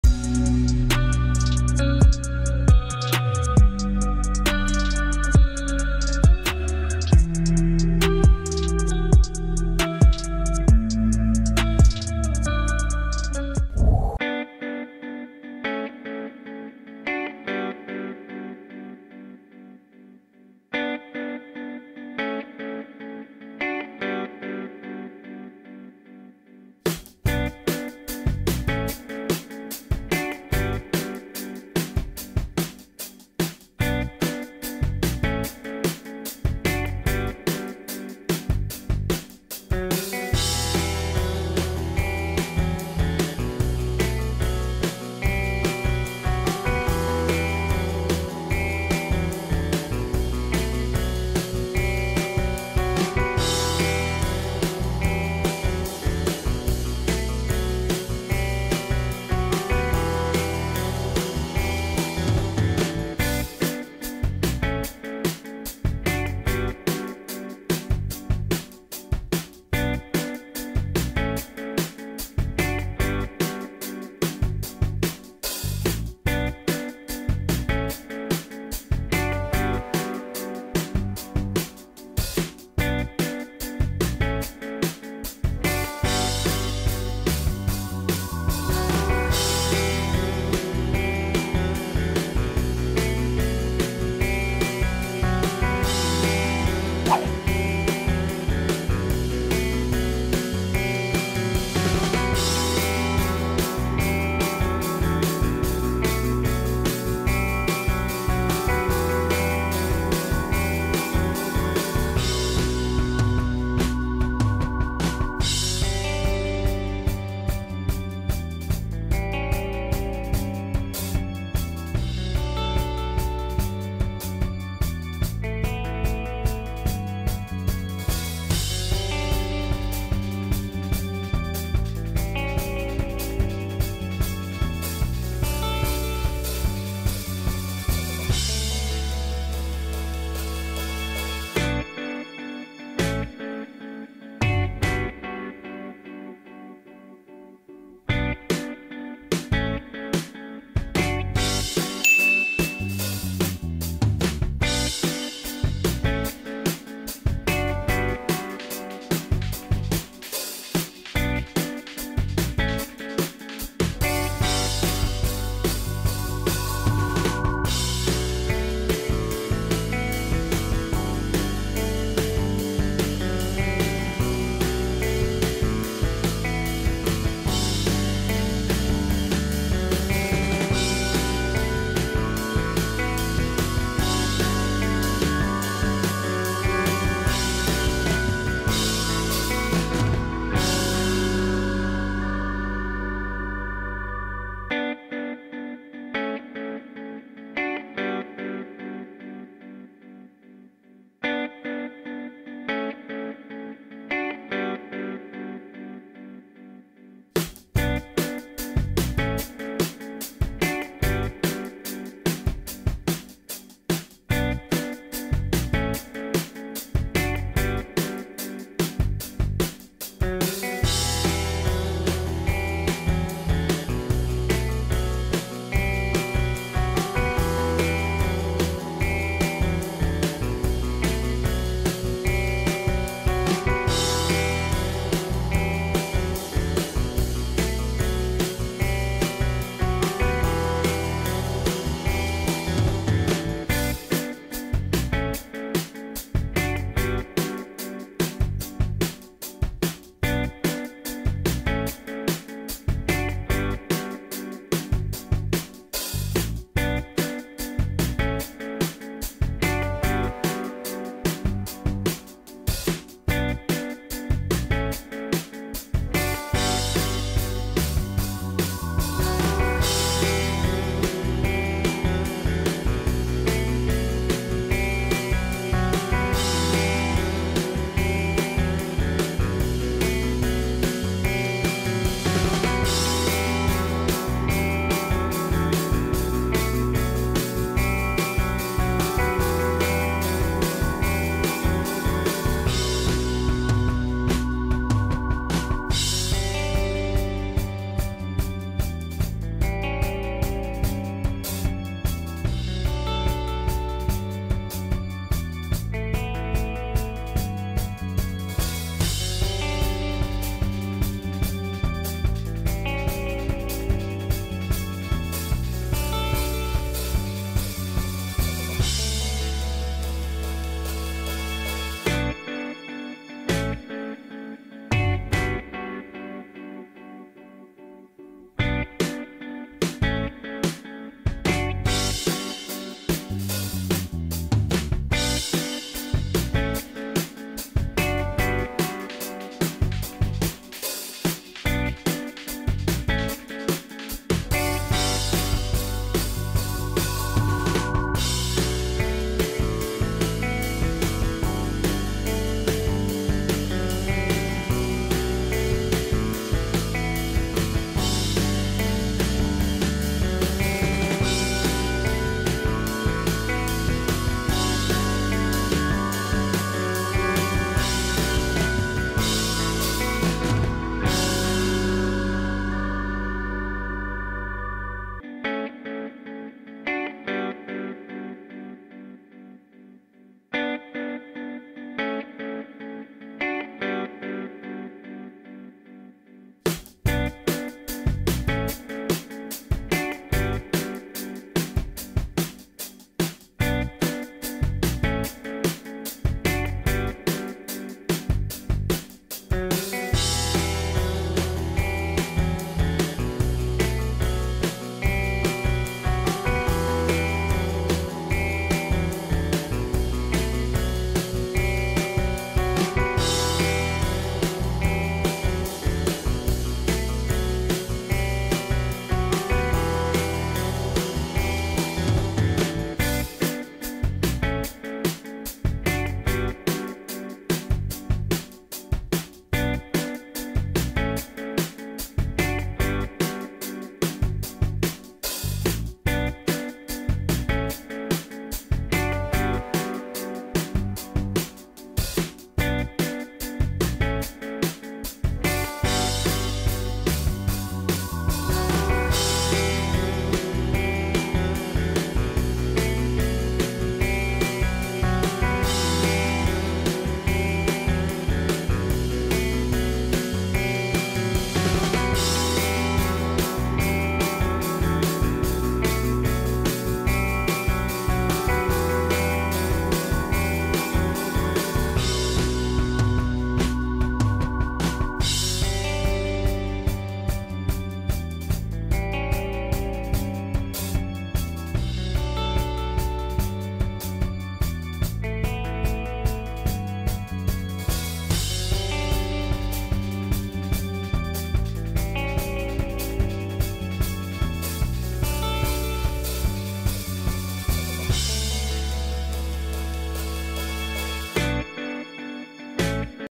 TEACHER OF THE YEAR CERTIFICATE DESIGN in MS Word
In this tutorial, I show how to create a certificate for Teacher of the Year. Teachers deserve recognition, so I encourage you to use this design to make your teachers happy.